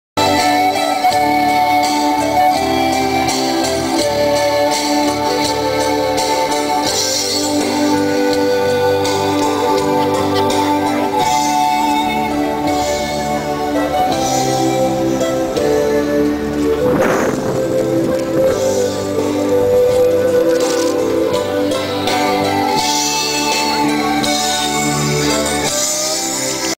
Música regional no centro de Buenos Aires
Índios de algum lugar mostrando sua arte no Centro de Buenos Aires.